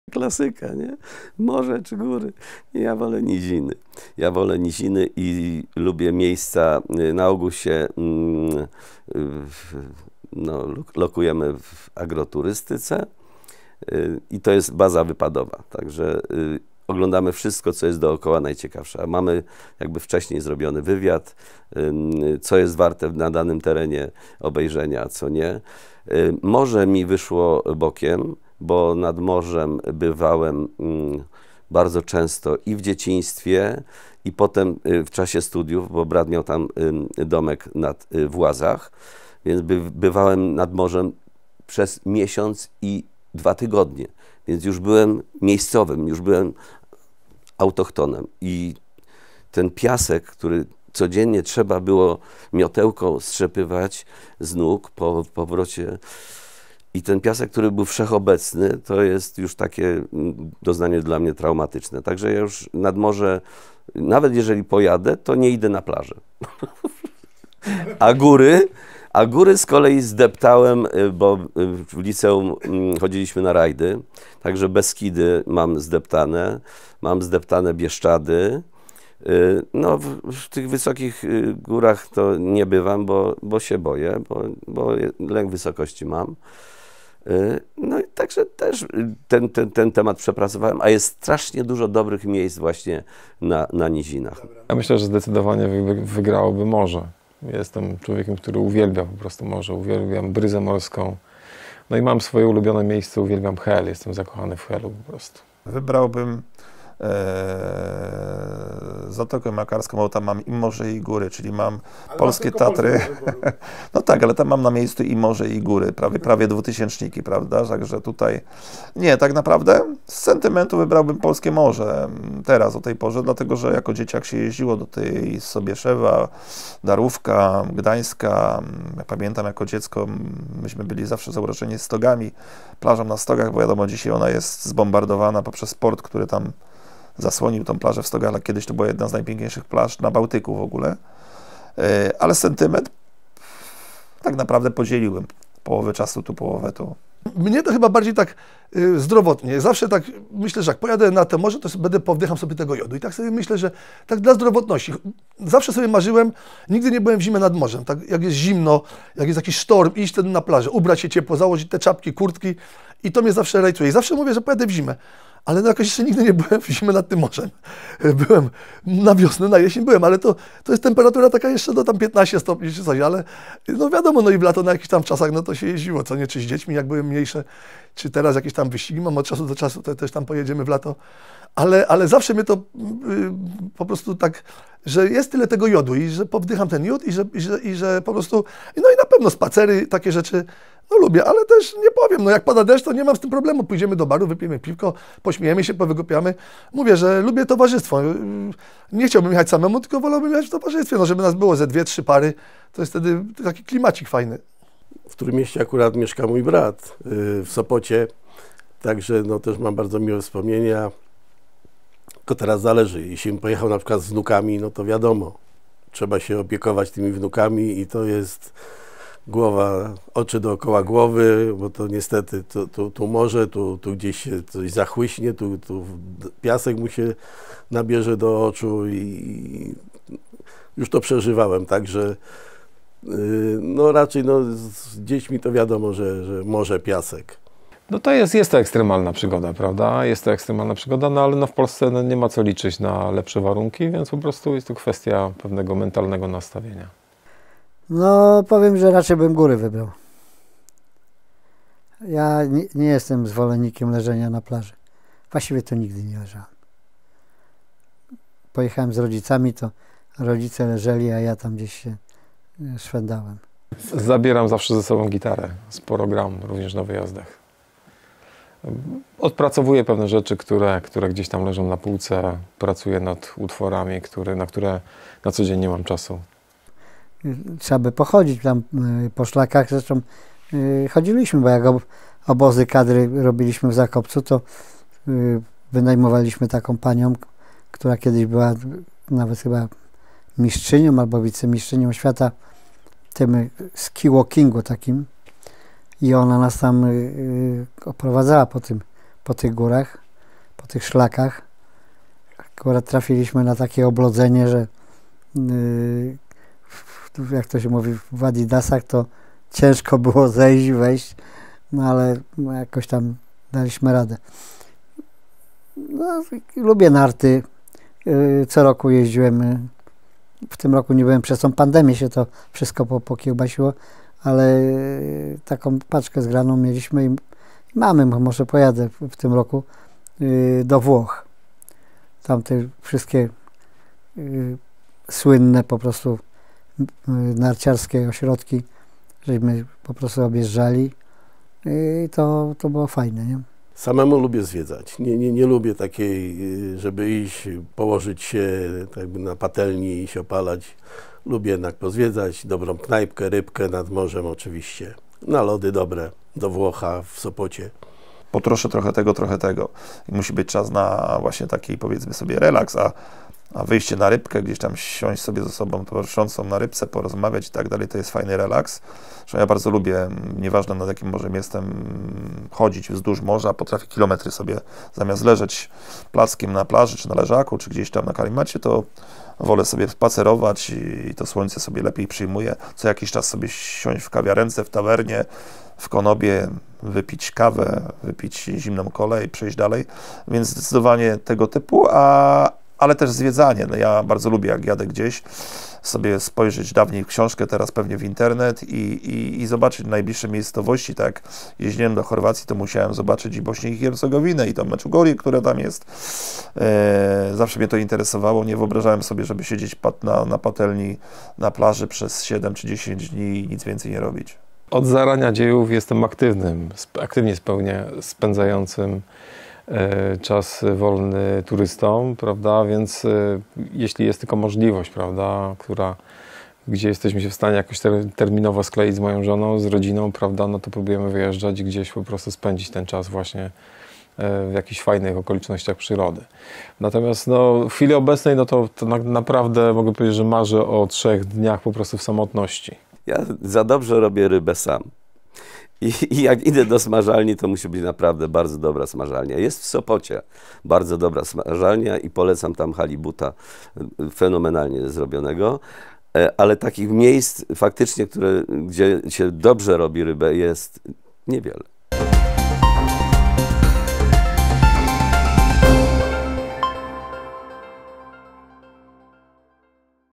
Moje Naj: Wakacje nad morzem czy w górach?
Gdzie spędzić wakacje nad morzem czy w górach? Na to pytanie w naszej sondzie odpowiadają m.in. Marek Cieślak były trener reprezentacji Polski na żużlu, Stefan Rybicki zegarmistrz, Robert Gliński gitarzysta Chłopców z Placu Broni, Antoni Rot aktor, Grzegorz Gronkiewicz były Mistrz Polski w kolarstwie.